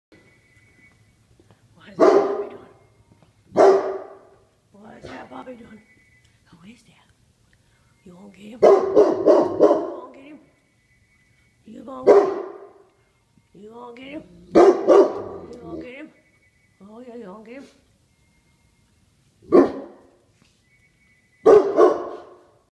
Bouvier Door Guard
My scary Bouvier des Flandres guard dog Gus at the front door. AKC GRC CH War Bouviator I-Gustoff AKC HT ABdFC HIT TDD ABdFC VBX GCG. Check us out on Facebook and Instagram as Bouviers From War Bouviator.